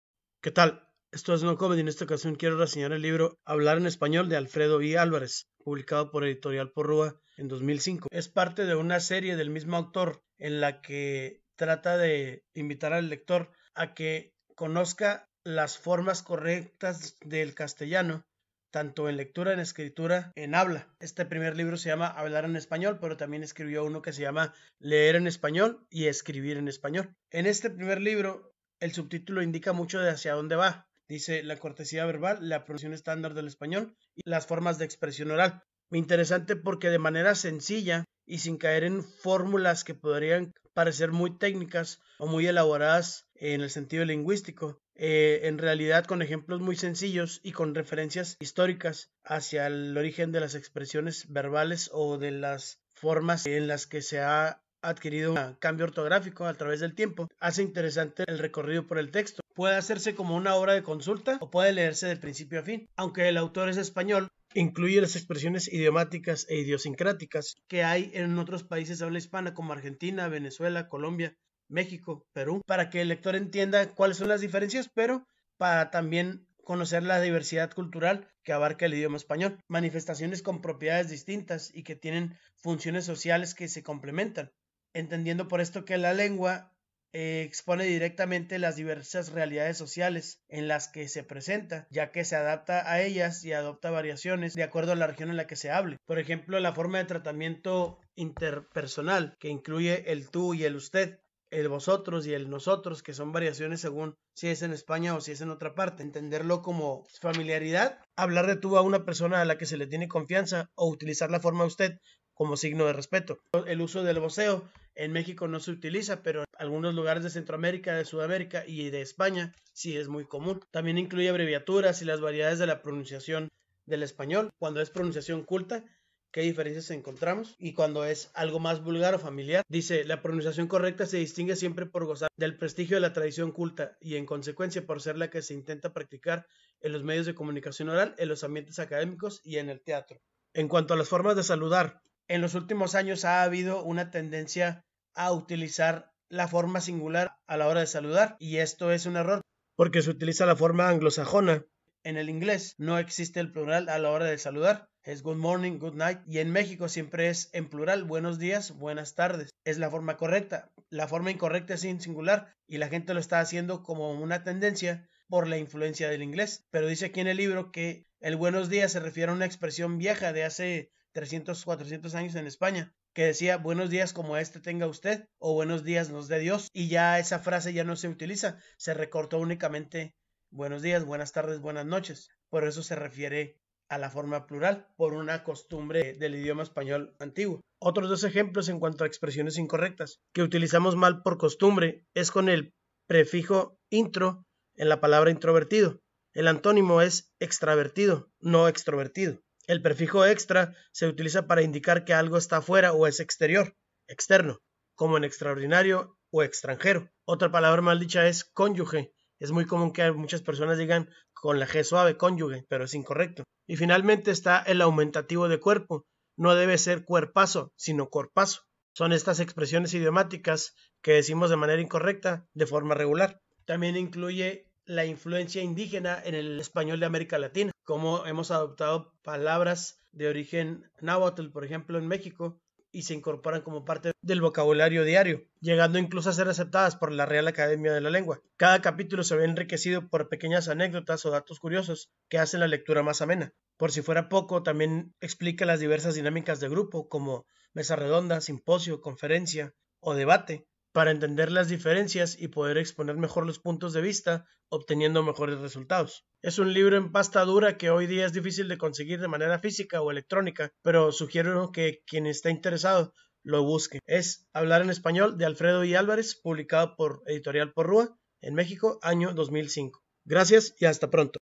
Libro: Reseña de 'Hablar en español' (Alfredo I. Álvarez)
El lingüista español Alfredo I. Álvarez se dio a la tarea de elaborar 3 textos para divulgar y fomentar un mejor uso del idioma castellano. Este primer libro se ocupa de las formas gramaticales más comunes y su uso, el vocabulario, conjugaciones y palabras mal empleadas, así como las diferencias regionales en América Latina y España.